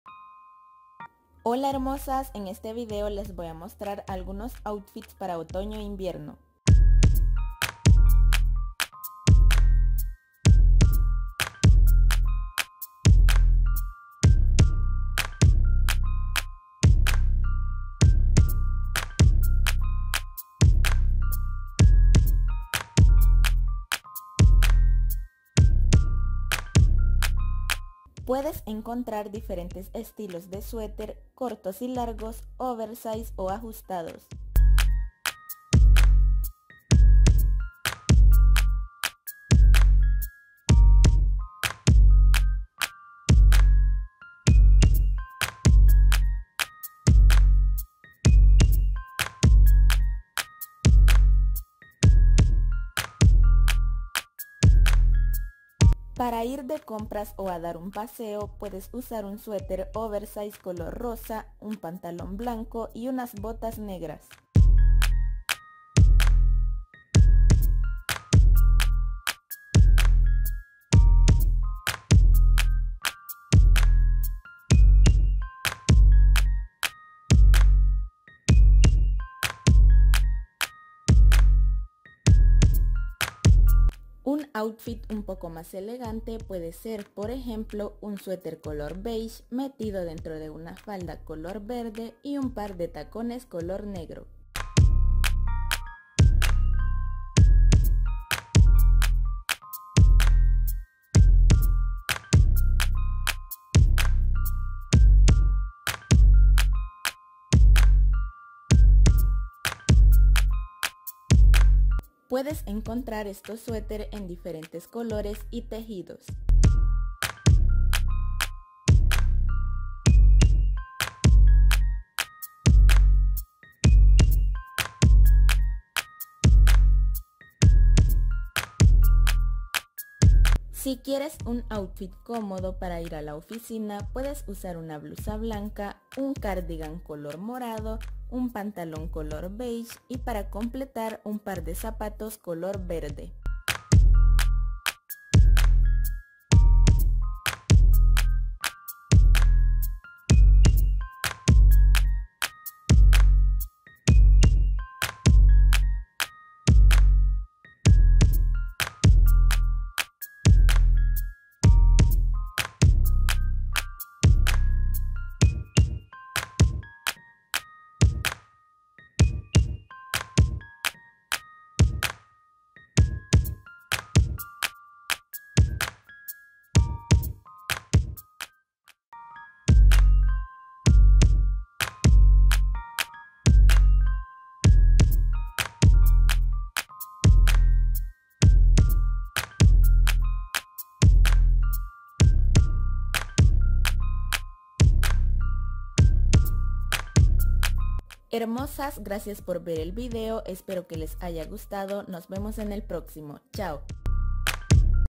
BELLOS OUTFITS DE MODA CON SUÉTER PARA OTOÑO/ INVIERNO 2021/2022!
Hola lindas, aquí les dejo este nuevo vídeo espero les guste!!!
Para los días de frío puedes crear muchos outfits con suéter a tu gusto y lucir siempre a la moda.
Hay diferentes prendas para la temporada de otoño invierno para que nunca pierdas el estilo.
Gracias y besos!!!